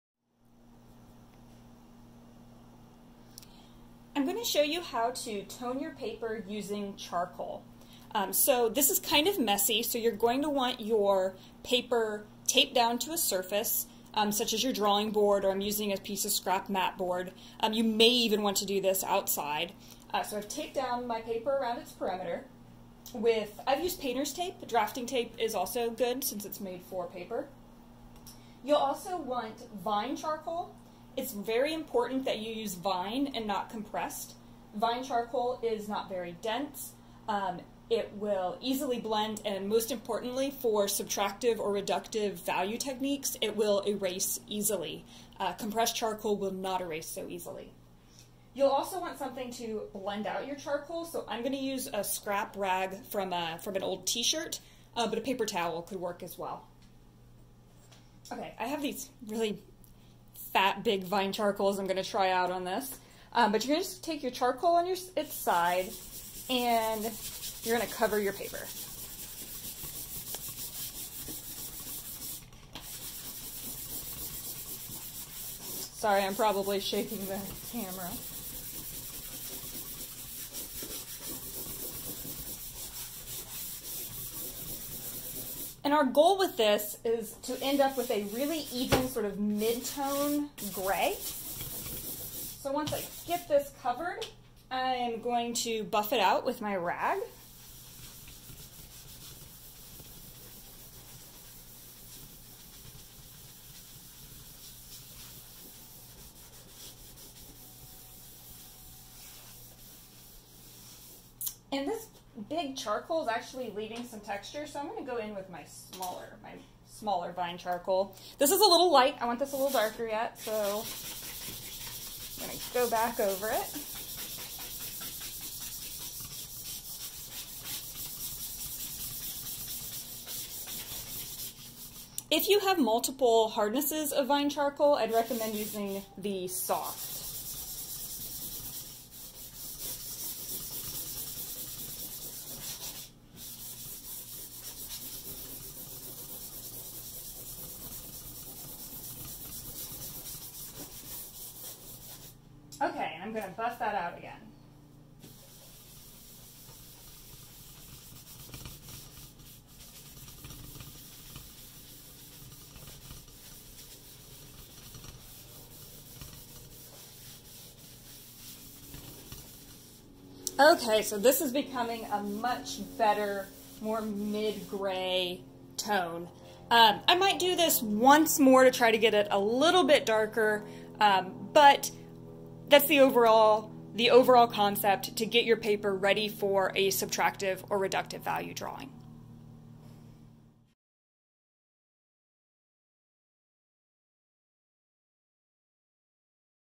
Creating Toned Paper for Subtractive Drawing
How to create toned paper for subtractive drawings using vine charcoal